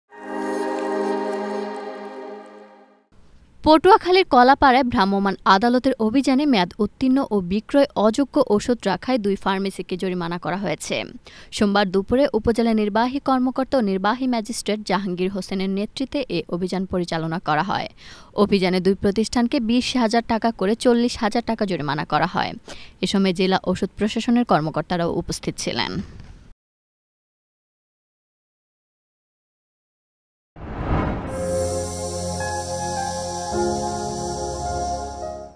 পটুয়াখালীর কলাপাড়ায় ভ্রাম্যমান আদালতের অভিযানে ২ ফার্মেসীকে জরিমানা| Channel S News | Channel S 2023
পটুয়াখালীর কলাপাড়ায় ভ্রাম্যমান আদালতের অভিযানে মেয়াদ উত্তীর্ন ও বিক্রয় অযোগ্য ঔষধ রাখায় দুই ফার্মেসীকে জরিমানা করা হয়েছে। সোমবার দুপুরে উপজেলা নির্বাহী কর্মকর্তা ও নির্বাহী ম্যাজিষ্ট্রেট.

1: News :
2: Entertainment News :
3: Sports News :